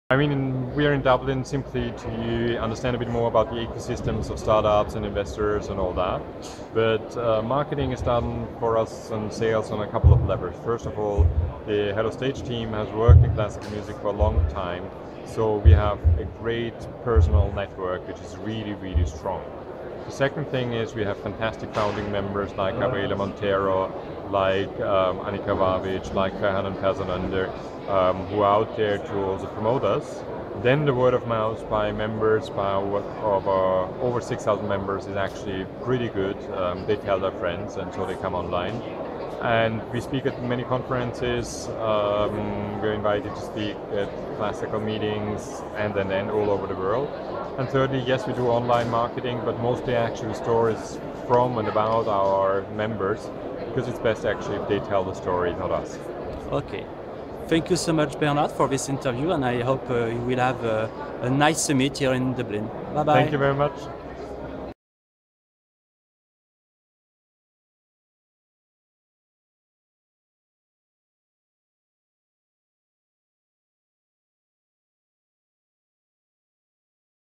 Opera-digital.com
Promotion Strategy of HelloStage, a social professional platform dedicated to classical music and opera